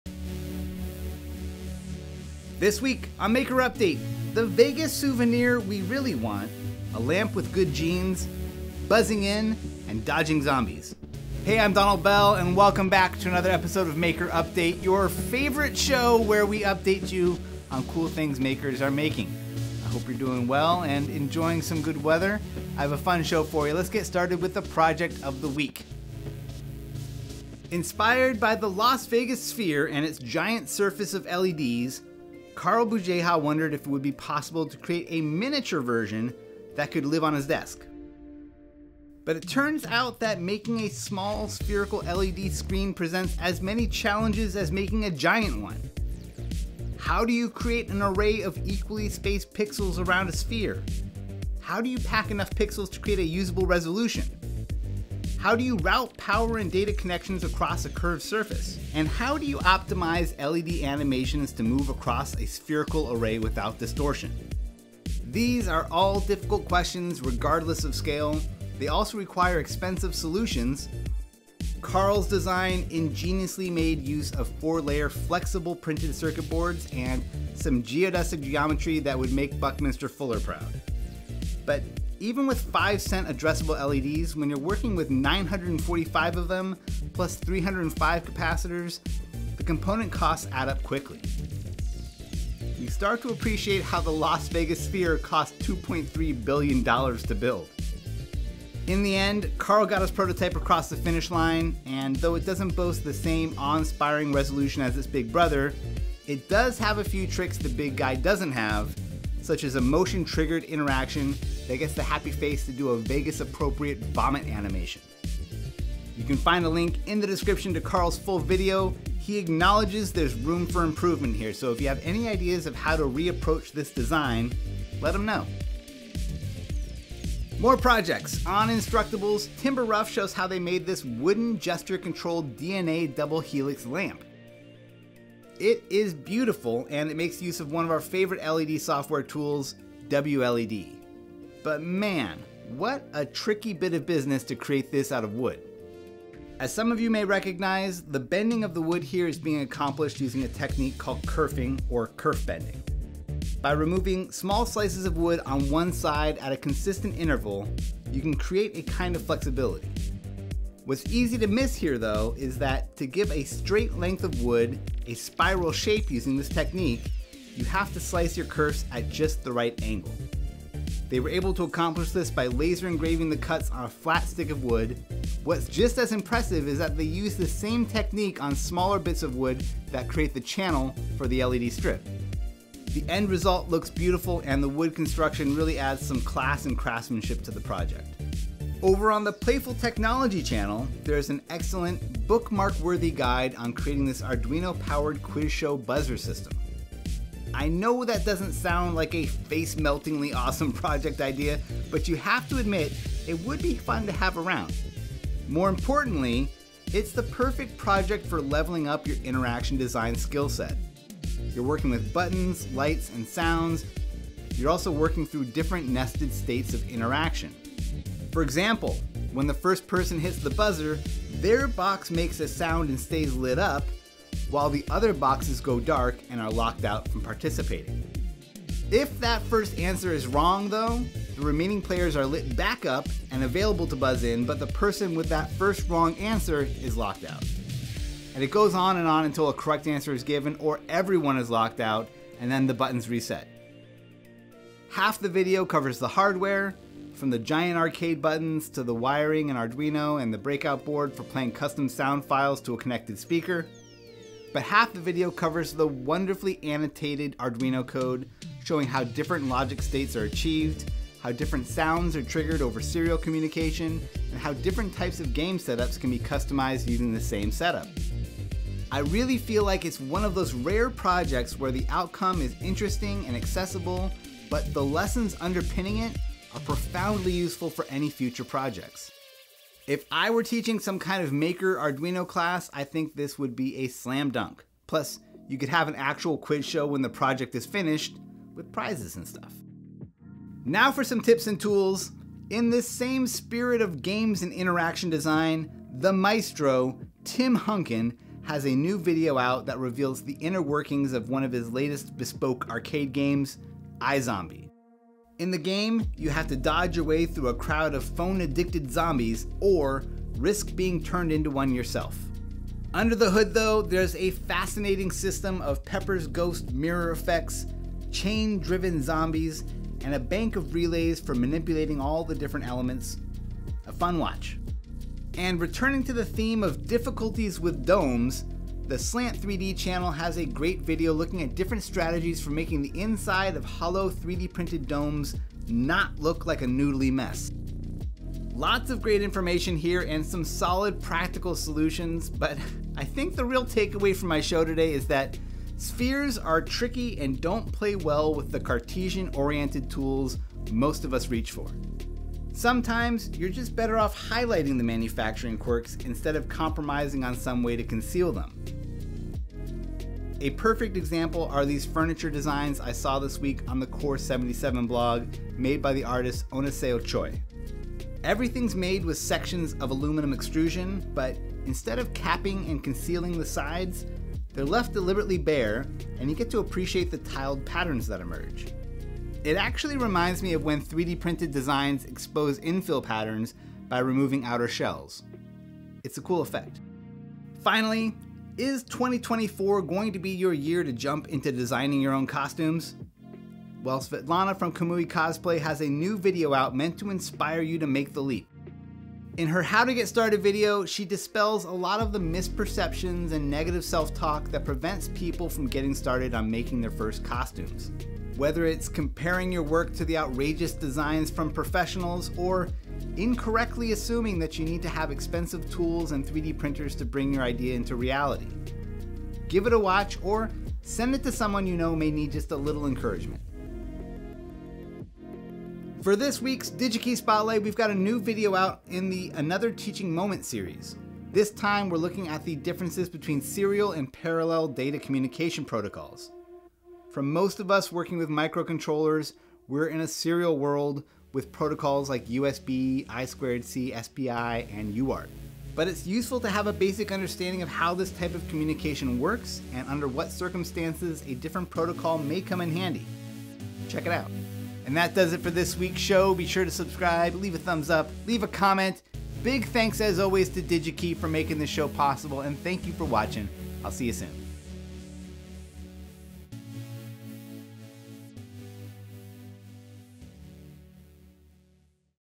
Vegas Baby [Maker Update] | Maker.io
This week on Maker Update: the Vegas souvenir we really want, a lamp with good genes, buzzing in, and dodging zombies.

Building a MINI Vegas Sphere by Carl Bugeja 0:28

=More Projects=

Gesture Controlled DNA Wooden Desk Lamp by Timber Rough 2:06

DIY Arduino Quiz Buzzer System by Playful Technology 3:08

=Tips & Tools=

The Secret Life of Arcade Machines, i-Zombie Tim Hunkin 4:51

Secrets to Better 3D Printed Domes by Slant 3D 5:24

Furniture Made from Aluminum Extrusions by Oneseo Choi 5:51

How to get started with Cosplay in 2024! KamuiCosplay 6:27

=DigiKey Spotlight=

Serial vs Parallel Communication Protocols - ATM 7:06